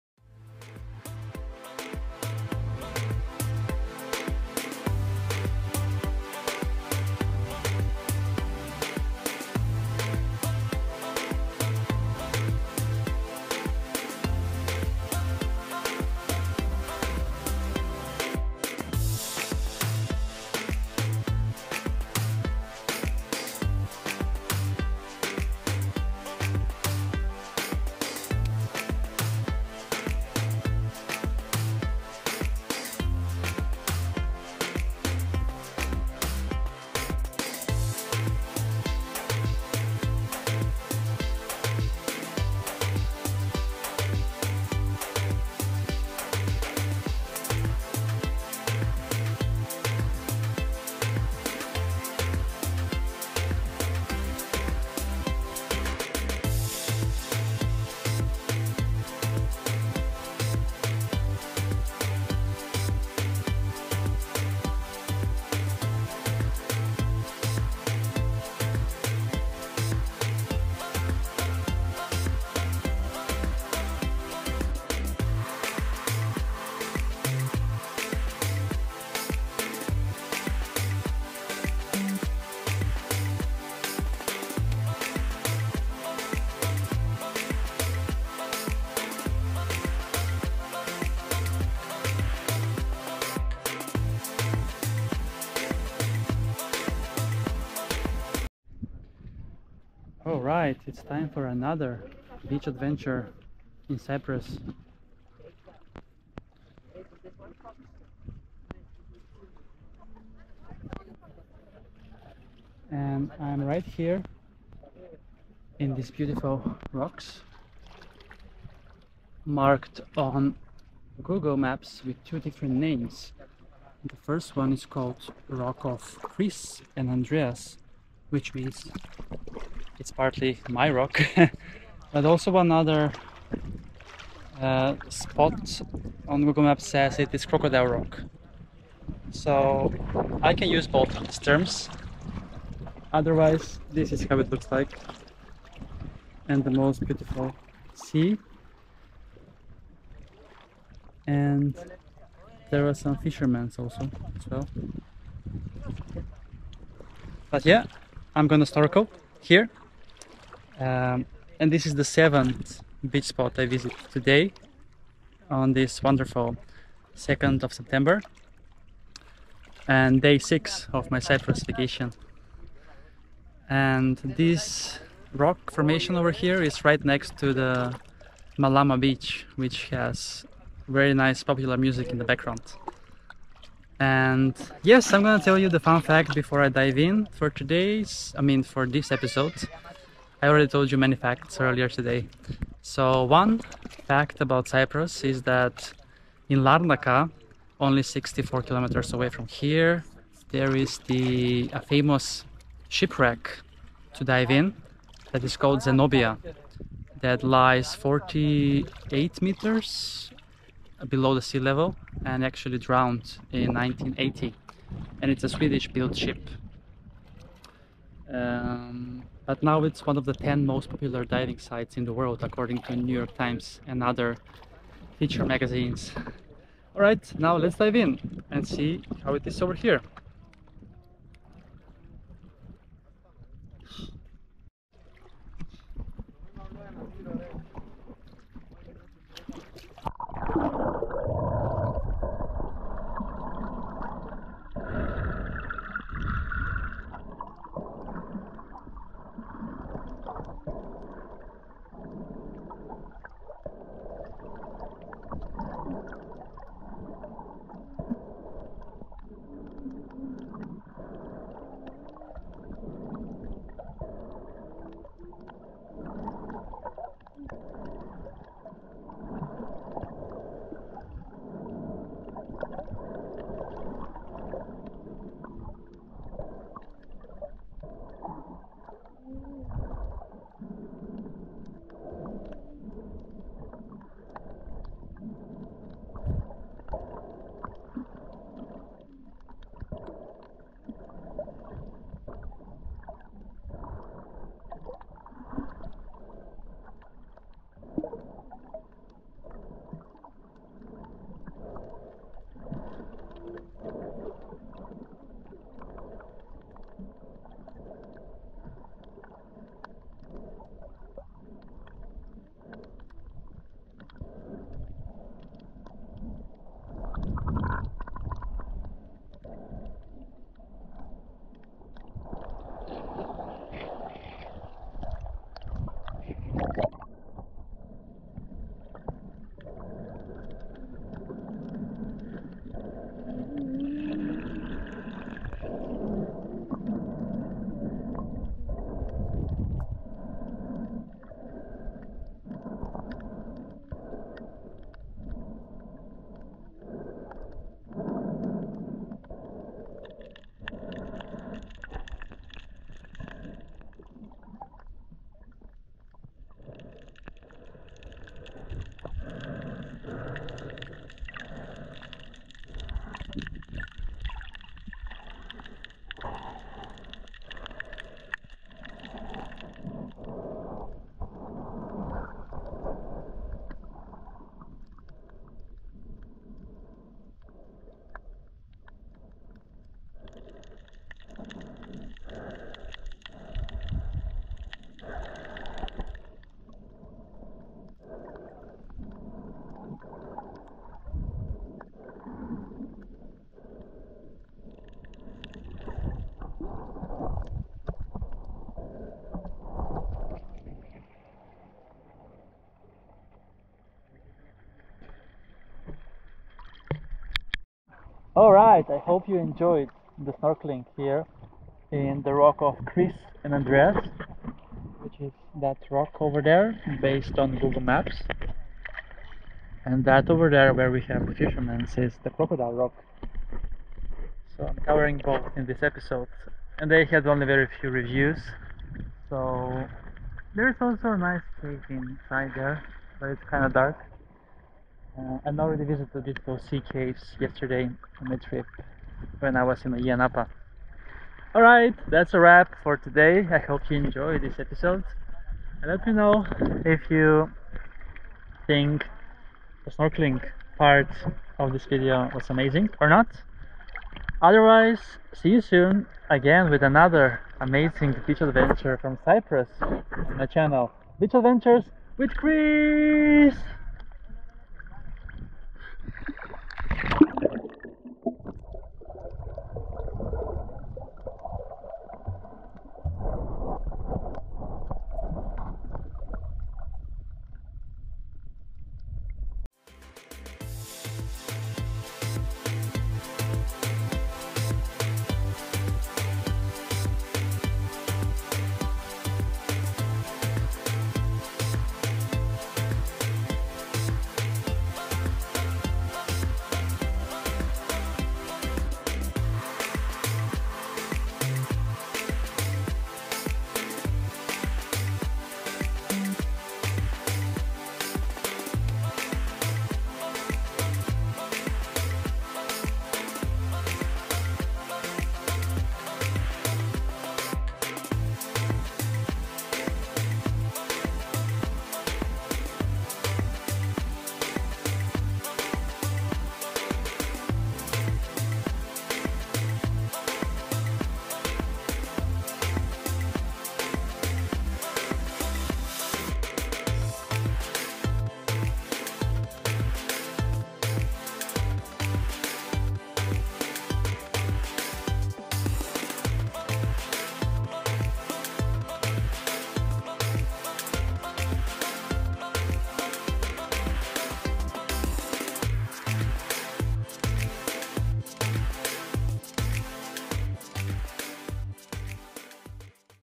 4K Snorkeling Adventure in Paralimni, Cyprus
Cyprus Vacay 2023: Time to continue exploring on Day 6 of my vacay!
I am enjoying in a beautiful snorkeling adventure at this scenic natural rocks named "Rock of Chris and Andreas". This place is located right next to the popular Malama Beach.

Adventure: Cyprus Vacay 2023
Episode: #047
Day: 6
Date: 2 September 2023
Outdoor Temp: 29°C
Water Temp: 30°C
Location: Rock of Chris and Andreas, Paralimni
Sea: Mediterranean Sea
Camera: GoPro Hero11 Black
Setting: 4K Video Wide (Speed 1x, 2x) + Google Earth
Edited in: CapCut